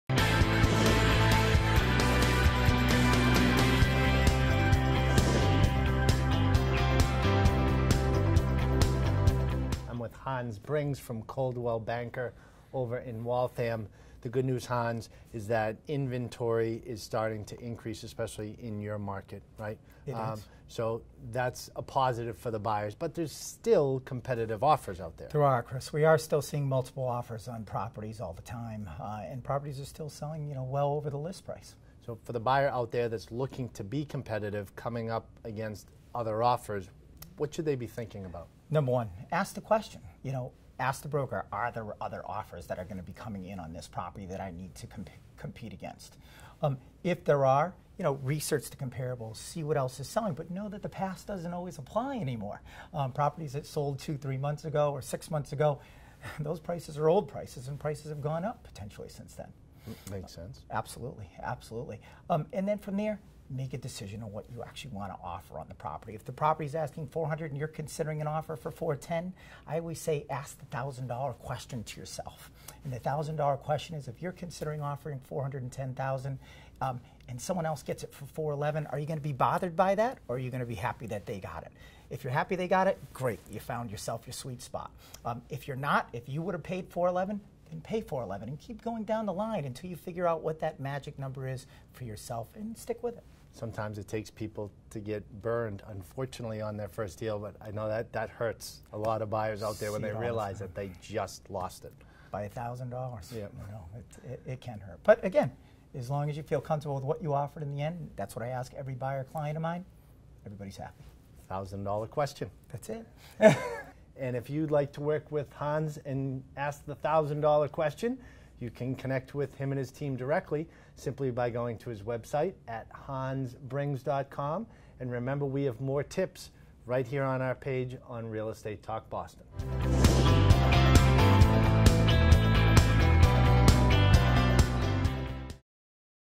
Buyers: Ask Yourself the $1000 Question
When you lose out on a home by a fraction of a bid, it hurts. But if you wouldn’t have raised your offer, even by $1000, then just stick with your bid and move on to something better that’s within your price range.